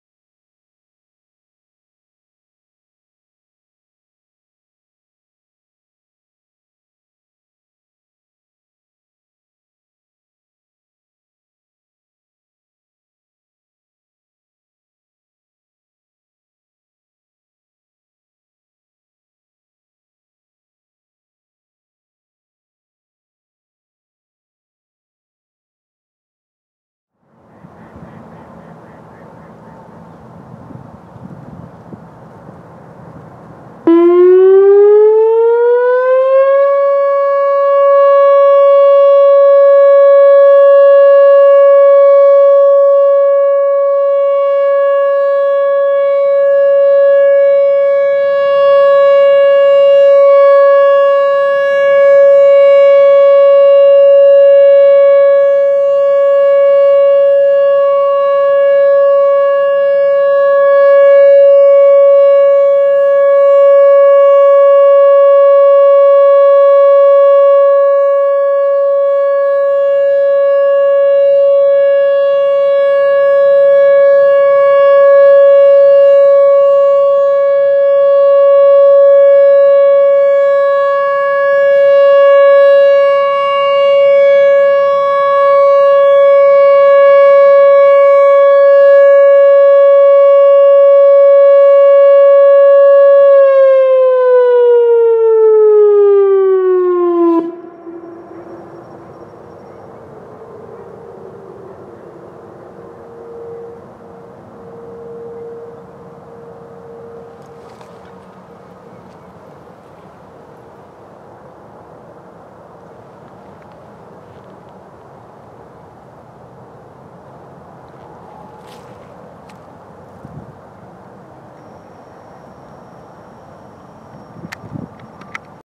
Whelen Vortex R4 | Alert | Craig Beach, OH | 12/15/18
Well, third time's the charm! I went down to Mahoning County again this weekend despite the threat of rain after a disappointing string of failures from them over the past two weeks, and not only did it not rain as it was supposed to, but my risk paid off and the siren sounded!

This particular Whelen is located at Lake Milton State Park at the Robinson Point Boat Launch site (1357 Jersey Street, Craig Beach) and replaced a Model 5 which is disconnected and down the street at the VFD. This had to be one of the quietest sirens I've ever heard, even with the 560hz tone set; although these sirens do carry quite far. This is one of the county's newer Whelens; this one was installed around 2014. The sort of shaky vibrating noise was not the microphone or my camera, that was coming from the siren itself. I'm not sure if that's the screen vibrating or one of the drivers is wearing out.

Mahoning County consists of mostly all Whelens (Vortexes, WPS-4004s, and OmniAlert OA-4’s as new sirens since 2015), with one active Thunderbolt 1000 in downtown Youngstown (near a Whelen 2905), with the Whelens making up the rest of the active system. Mahoning County/Youngstown tests their 52 sirens countywide every Saturday at noon, year-round, weather permitting. Enjoy!